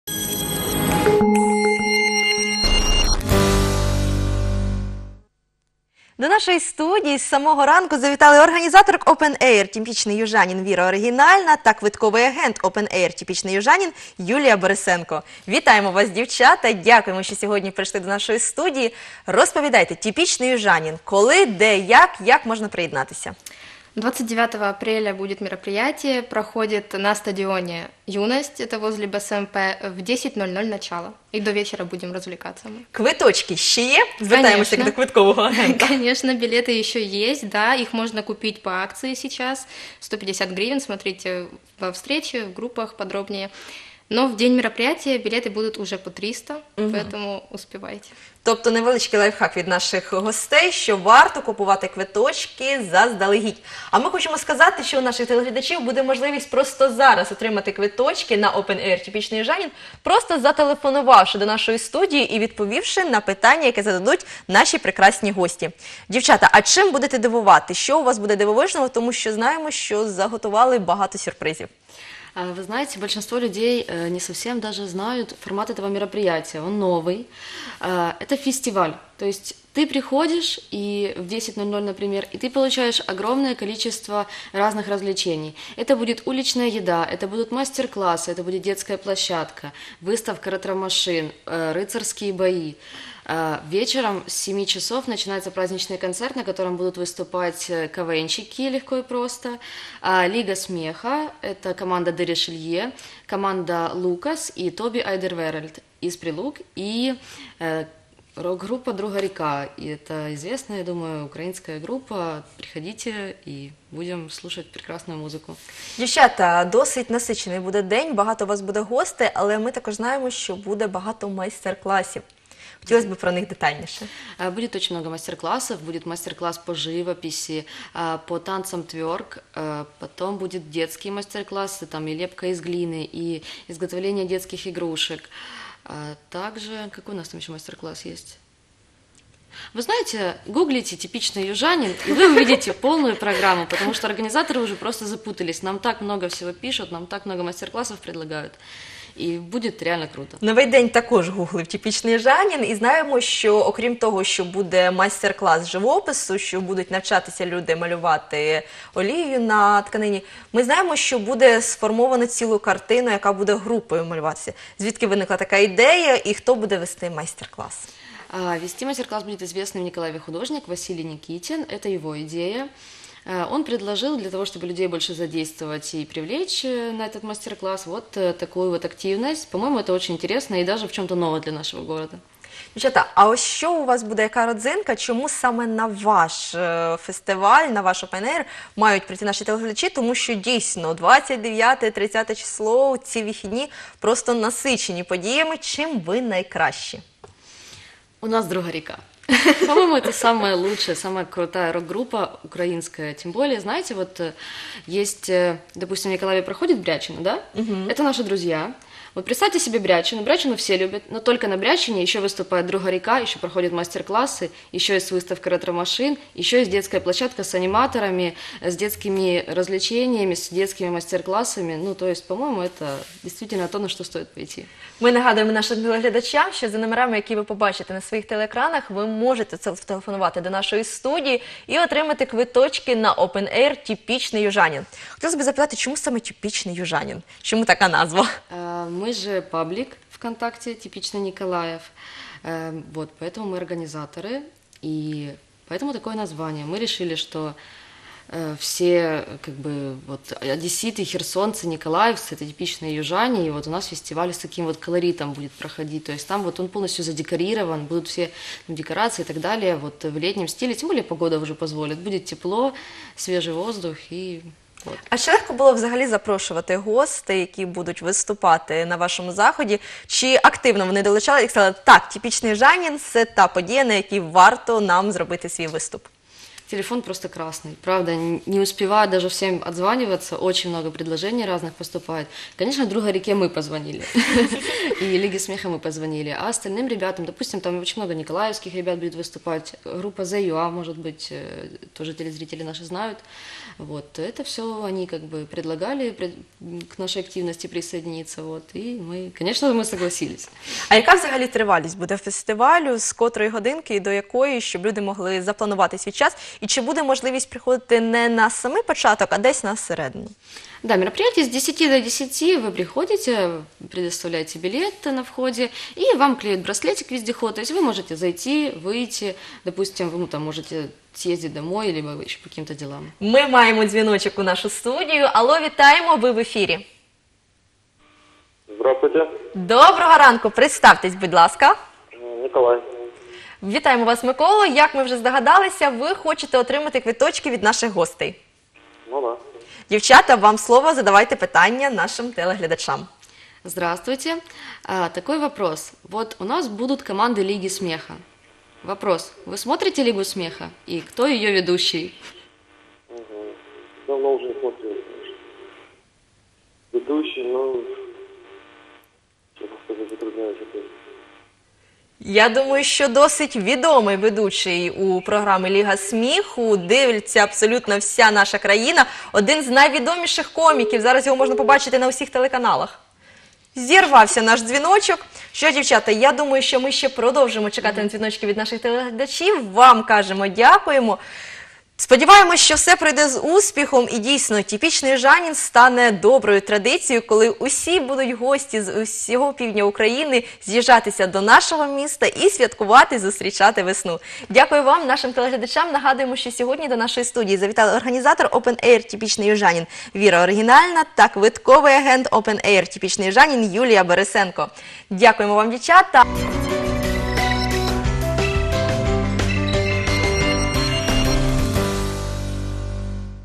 Новий день. У Миколаєві відбудеться фестиваль «Типичный южанин»
Гості студії - Віра Оригінальна та Юлія Борисенко.